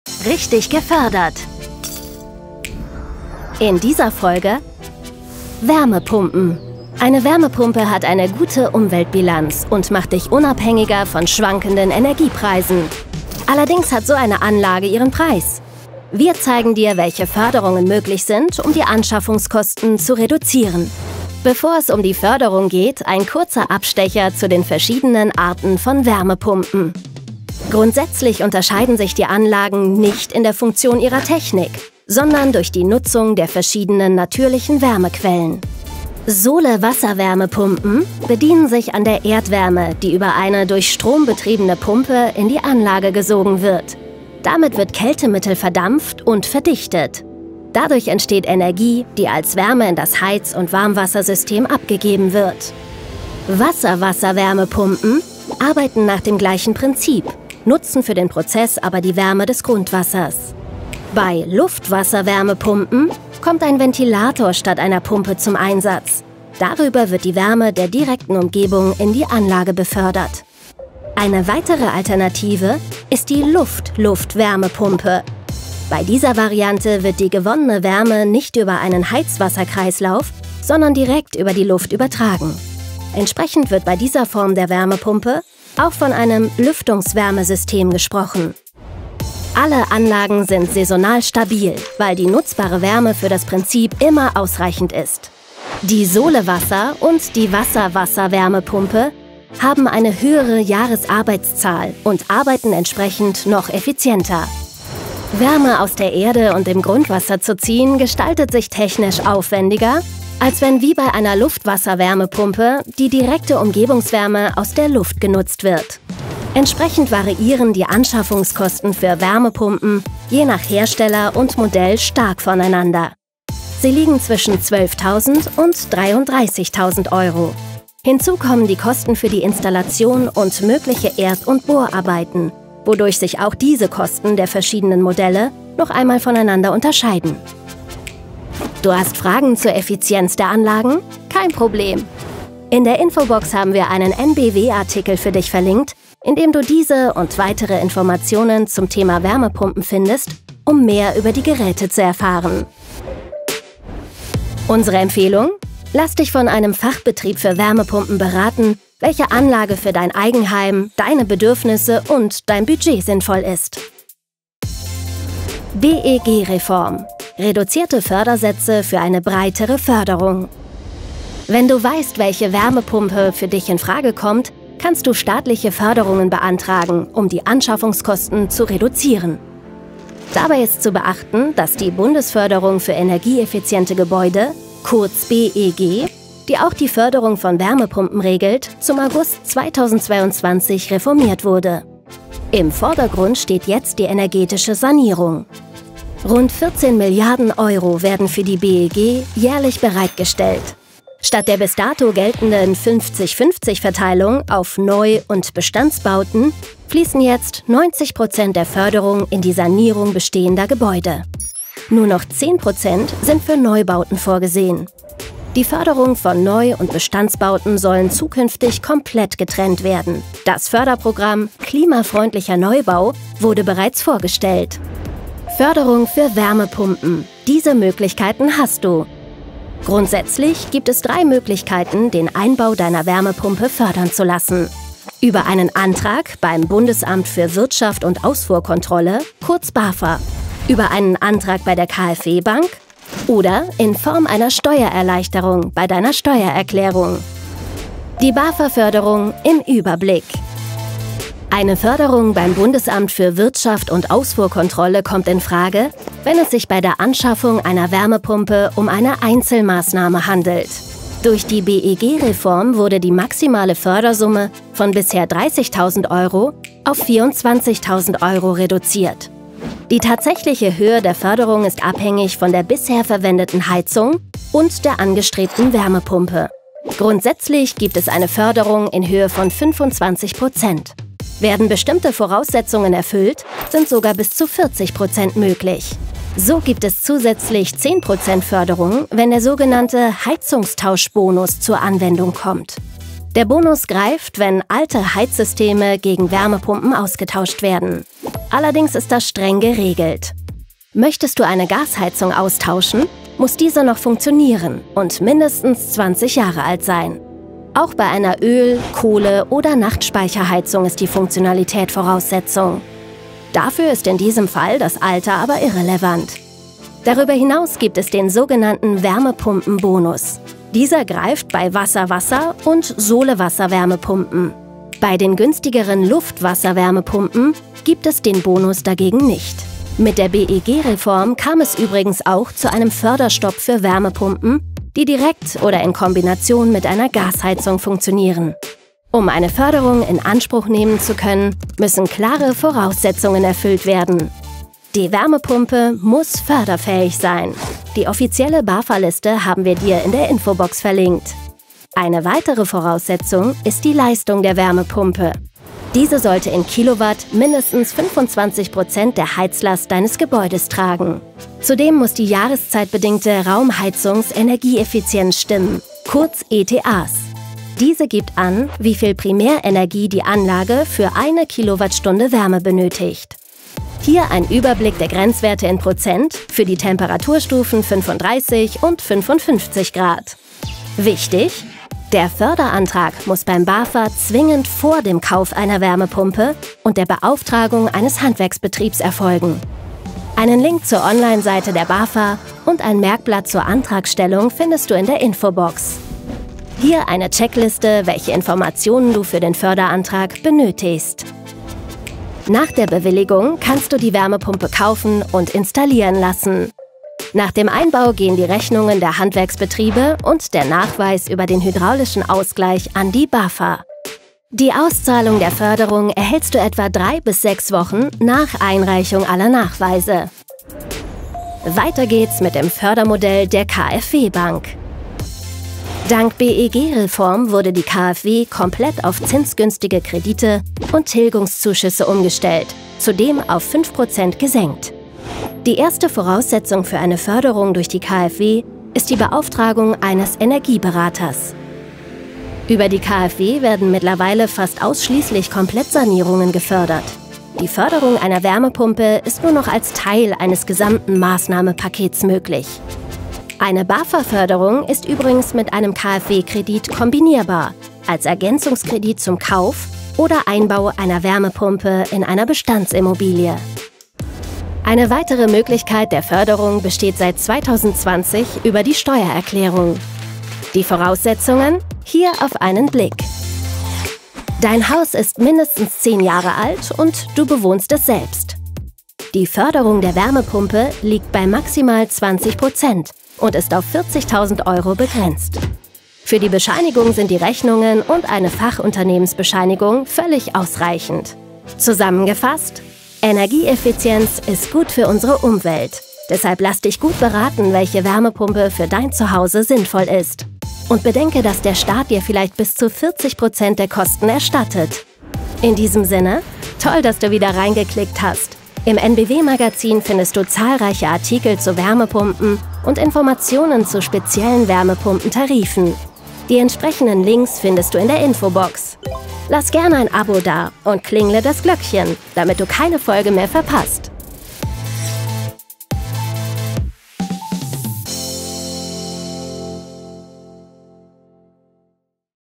Wärmepumpe -Förderung erhalten und Investition noch lohnender machen 💵⚡ | Richtig gefördert - EnBW
Willkommen zu einer neuen Folge von "Richtig gefördert"! In diesem Video zeigen wir dir alles über die Förderung von Wärmepumpen und wie du in Deutschland von diesen Förderungen profitieren kannst. ⚡💵

Wir zeigen dir die verschiedenen Wärmepumpentypen und ihre Funktionen. Dabei schauen wir uns die Sole-Wasser-, Wasser-Wasser-, Luft-Luft- und Luft-Wasser-Wärmepumpen genauer an und erklären dir die Funktionsweisen und Unterschiede, damit du die richtige Wahl für dein Zuhause treffen kannst.

Im weiteren Verlauf beleuchten wir die Fördermöglichkeiten der Wärmepumpen. Dazu gehen wir auf die Förderung der BAFA ein, betrachten die KfW-Kredite und erklären, inwiefern sich die Anschaffung einer Wärmepumpe über die Steuererklärung fördern lässt.

Erfahre, wie du finanzielle Unterstützung erhältst und deine Investition noch lohnender machst.

Mach dich schlau und nutze dieses Wissen, um die richtige Wärmepumpe für deine Bedürfnisse zu finden.

Wir zeigen dir:
🔍 Verschiedene Wärmepumpentypen und Funktionen
💵Unterschiedliche Fördermöglichkeiten
📃Wie du finanzielle Unterstützung erhältst

⏱️Kapitel
00:00 Intro
00:36 Sole-Wasser-Wärmepumpe
00:53 Wasser-Wasser-Wärmepumpe
00:58 Luft-Wasser-Wärmepumpe
01:11 Luft-Luft-Wärmepumpe
01:56 Die Anschaffungskosten
02:45 Die BEG-Reform
4:08 Die BAFA-Förderung
4:49 Der Heizungstausch-Bonus
05:15 Der Wärmepumpen-Bonus
07:01 Die Förderung durch KfW-Bank
07:44 Die Förderung über Steuerklärung

🔗 Links & weiterführende Videos:
Das könnte dich auch interessieren:

❓ Fragen?
Schreib uns in die Kommentare, ob du schon eine Förderung für deine Wärmepumpe hast, oder welche Themen du als nächstes sehen möchtest.

Bildquellen:
Getty Images
EnBW Website
EnBW Bynder